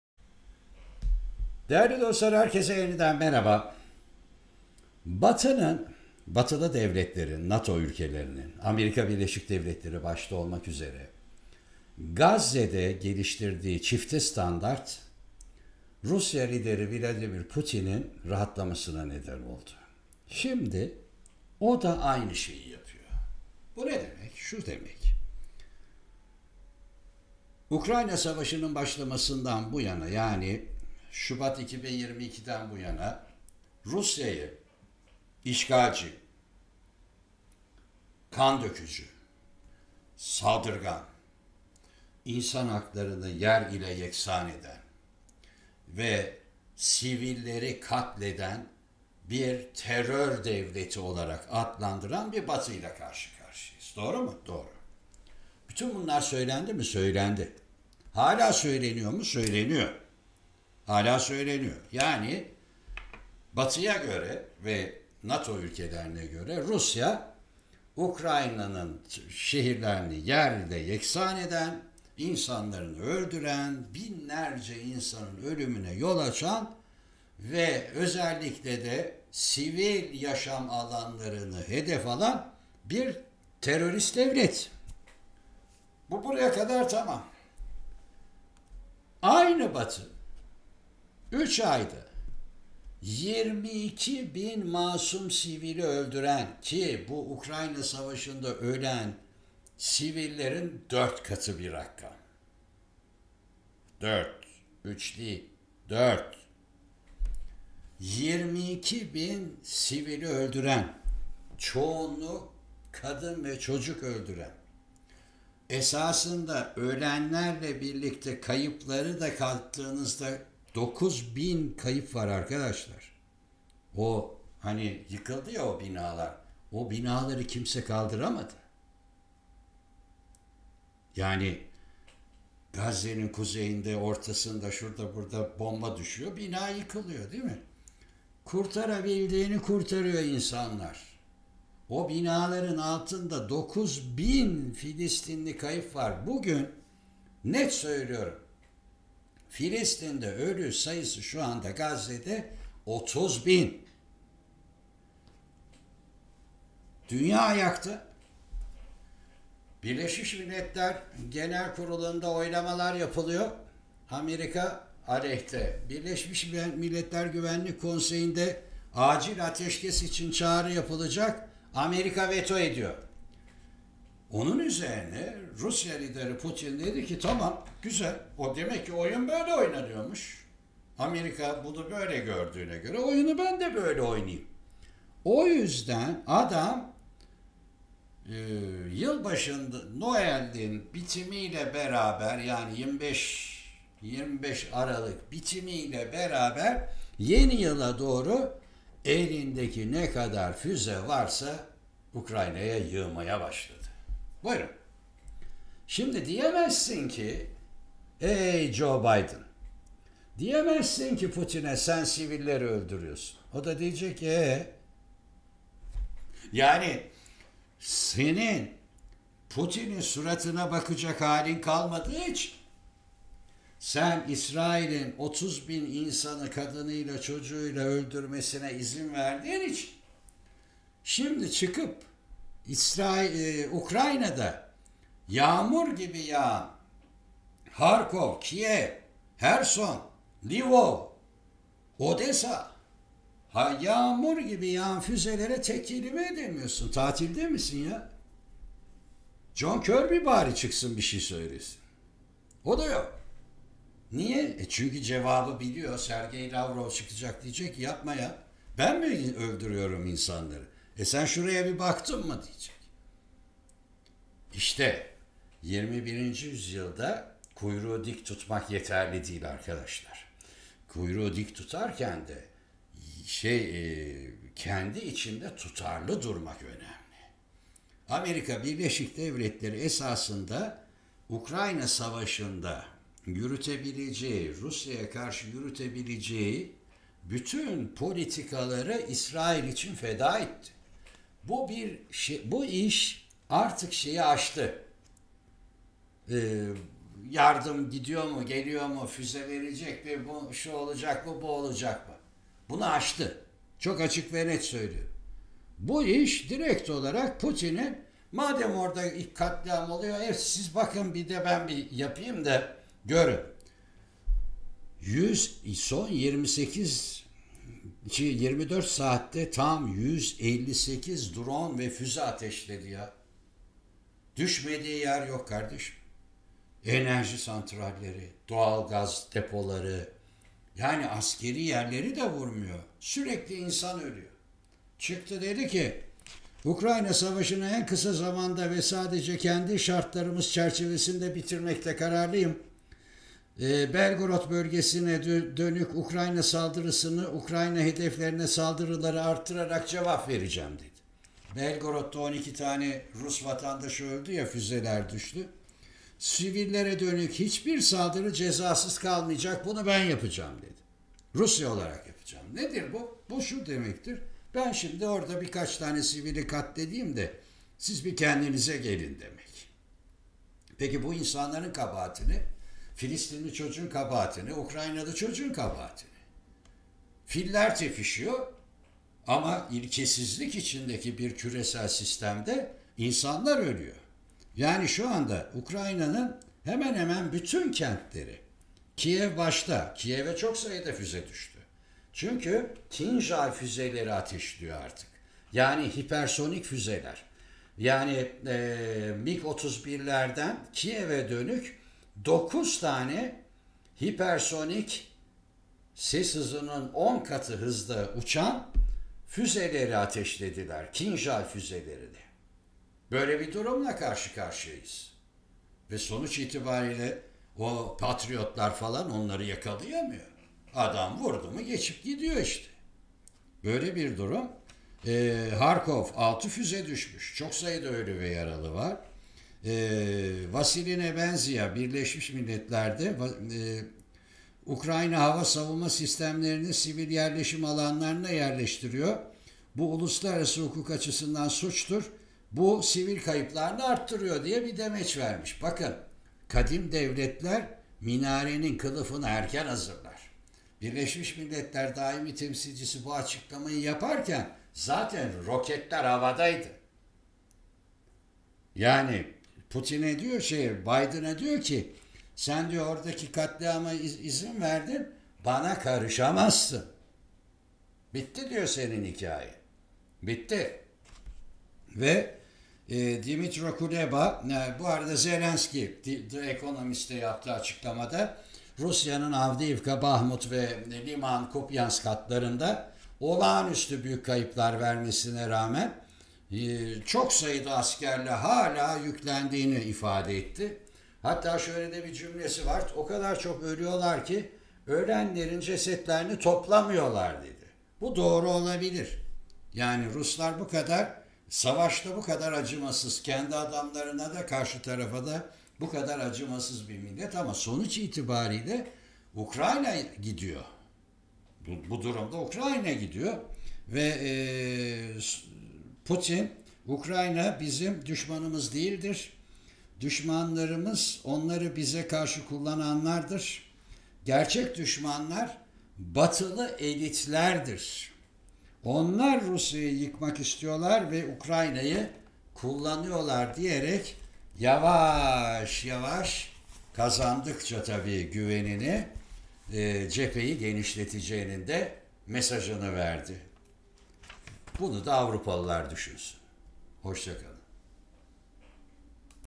UKRAYNA KANTLERİNE FÜZE YAĞIYOR!ZELENSKİ: HEDEFİMİZ KIRIM BATI KİEV'E BÜYÜK ASKERİ YARDIM HAZIRLIYOR
PUTİN BIDEN'IN GAZZE KATLİAMINA GÖZ YUMMASINDAN CESARET Mİ ALDI?
ABD artık Rusya'dan sivil saldırıların hesabını neden soramaz?
Rusya sivillere saldırarak Ukrayna'da çözülme mi yaratmak istiyor?